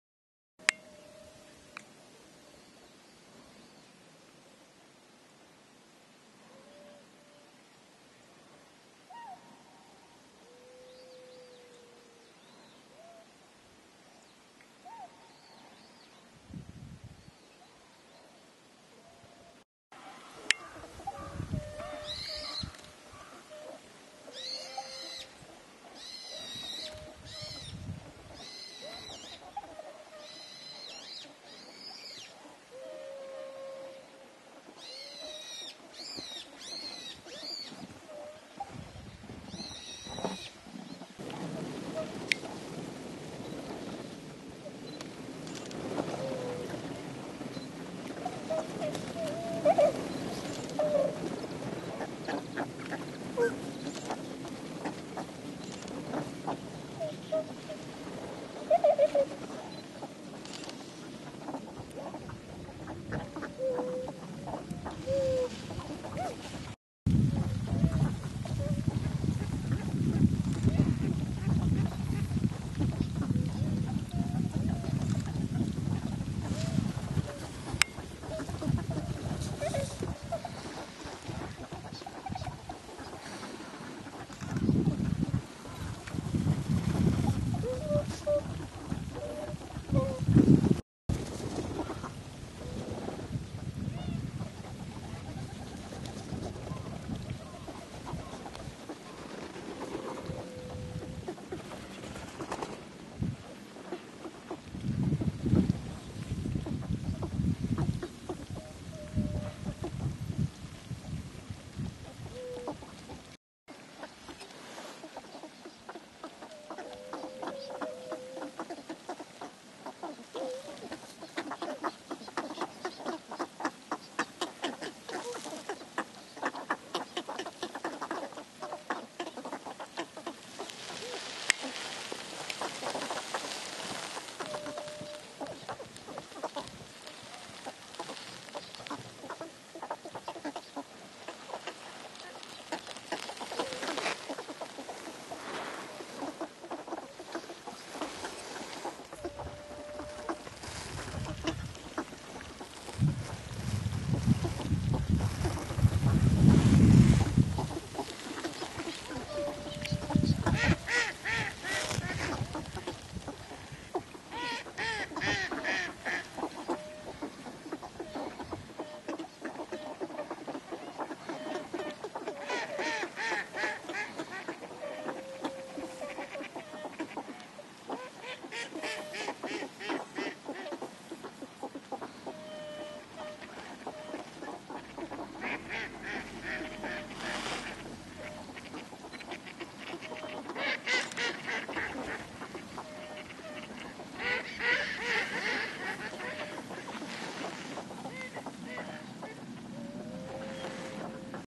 白鳥に会いに・・・大池（２） 福島県白河市
福島県白河市の表郷の白鳥飛来地に行ってきました。
でも、鴨の方が多いのカモ。